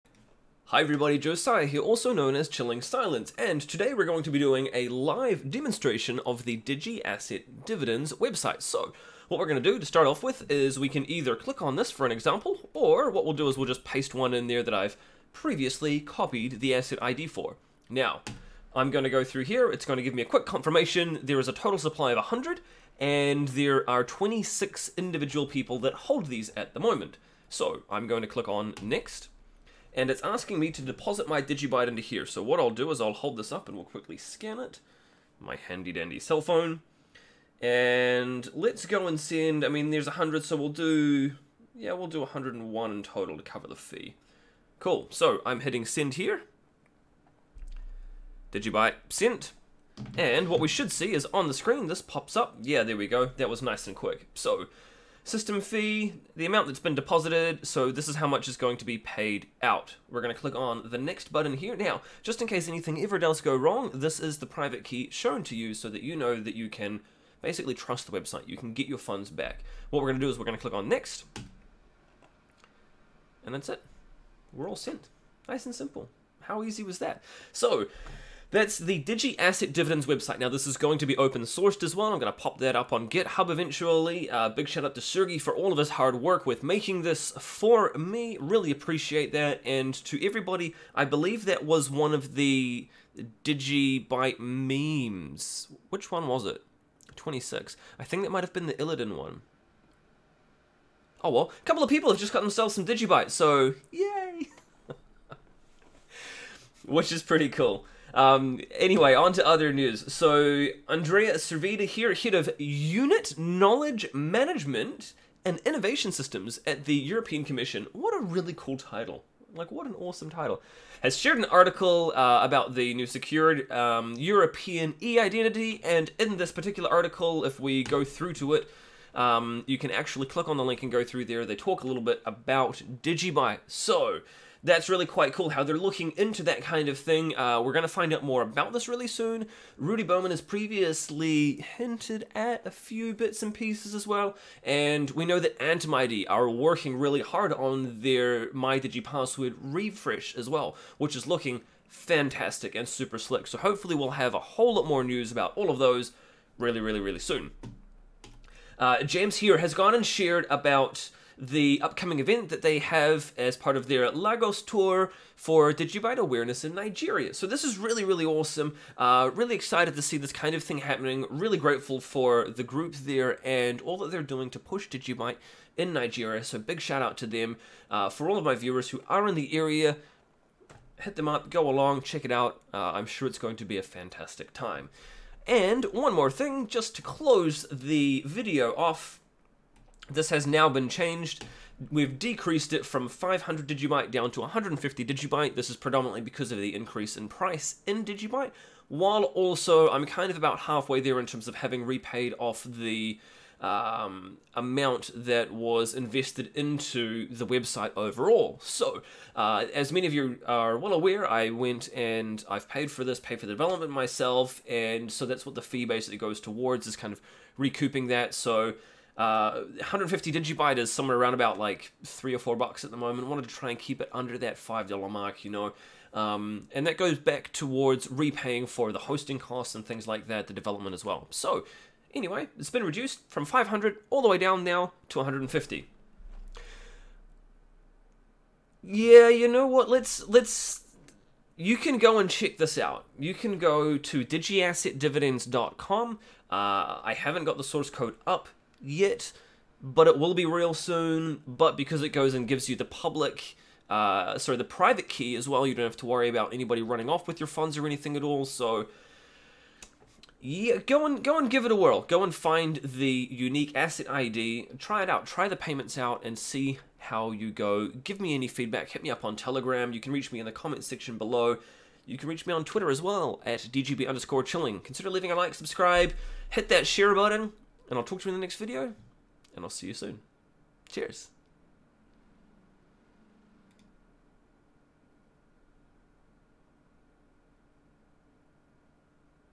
DigiByte Update - #218 - How badly can a live Dividends demo go?
Check out the Dividends website and flick through some feedback:
DigiAssetDividends.com

You can see that transaction go through too, with all the Outputs to all the holders:
e138ba4403ac09cf2af3a80f2f7257c93d876e8a9a554c9dbcfc491077ed1aab

Everyone gets an equal "share" based on the number of assets they hold, so whoever was holding 75% of those assets got 75% of the payout! (I believe that was Blazzord who created the asset)

We've got the DigiByte Awareness Nigeria Lagos Tour, talking Digital Payment Systems coming up:
Big thanks to James for organizing this, and great to see some fantastic looking speakers there too.

Check out the article about the European Commission looking in to e-identities, great to see DigiByte mentioned there too:

And last but not least we've got the reduced Asset Creation Fee over at CreateDigiAssets.com , it's now only 150 DGB so go give it a whirl!